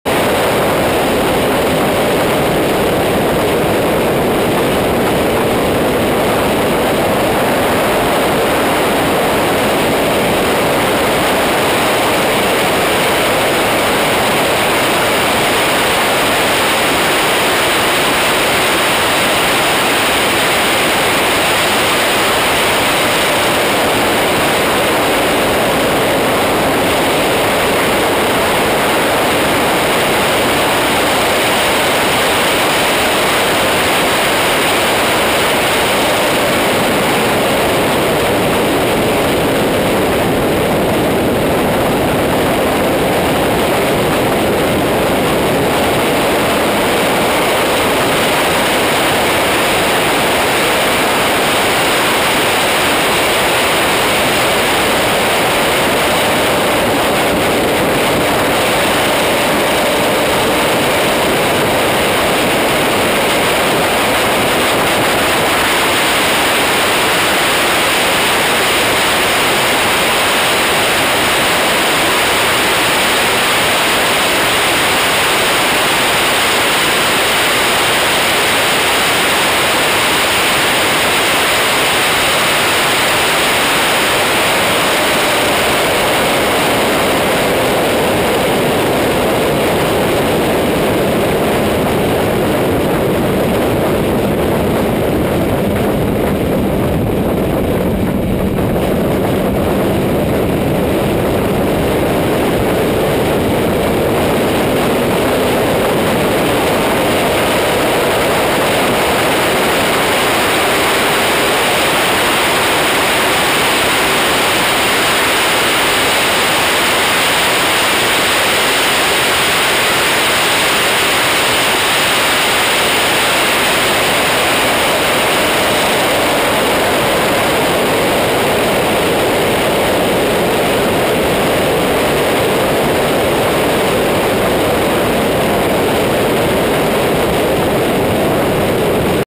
kawasaki zx10r 290km with passenger
Teo is behind me ... this was going 290 kilometers.